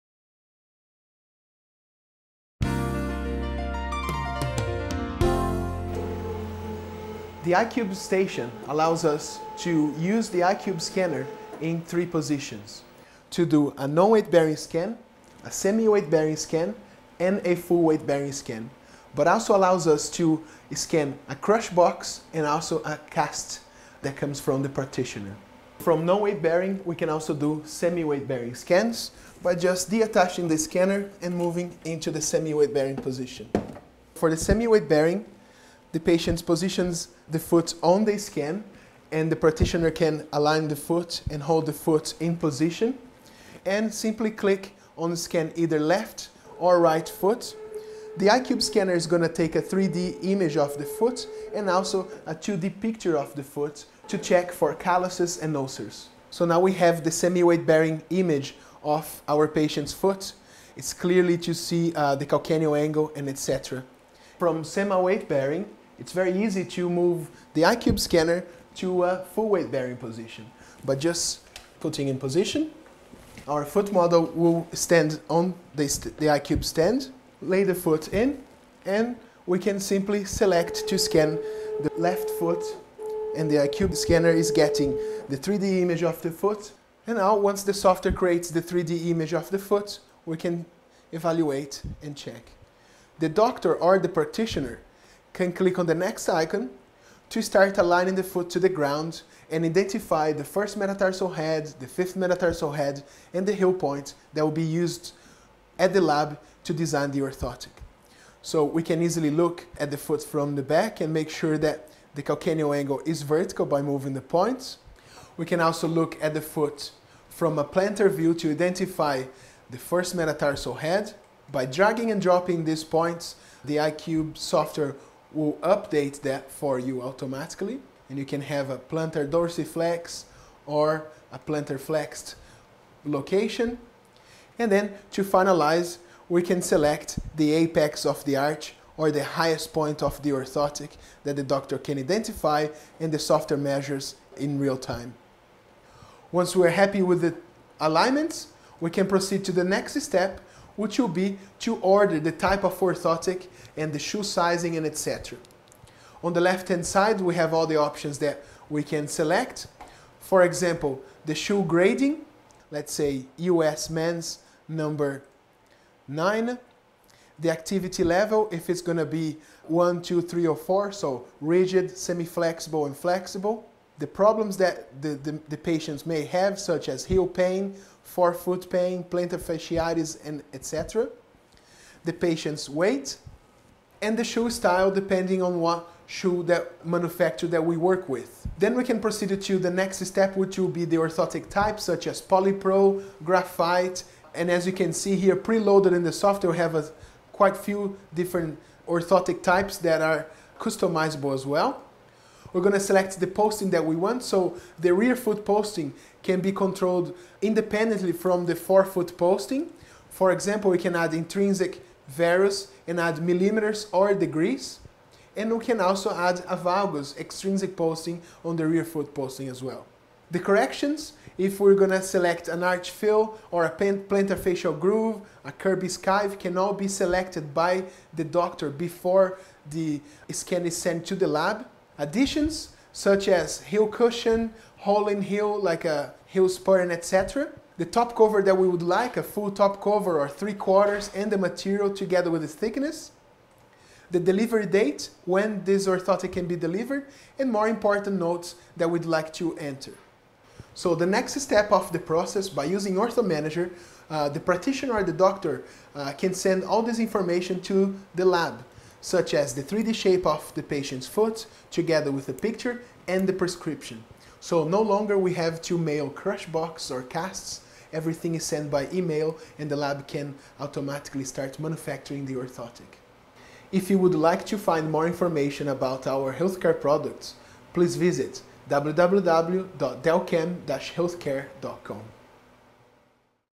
Delcam iQube Orthotic 3D Scanner Interface
Our new iQube foot scanner is designed to capture 3D digital models of a foot, cast or foambox. The foot can be capture in as weight bearing, semi-weight bearing and weight off. The scanner is based upon technology developed for the aerospace and automotive sector.

Whether you need to scan a foot a cast or a foam box impression, iQube can produce a high quality 3D image in seconds.

The design of the scanner allows your patients foot to be scanned in non- weight, partial-weight and full-weight bearing positions.

If you are foot practitioner, our portable iQube scanner allows you to remove the messy casting process and save money on expensive shipping costs.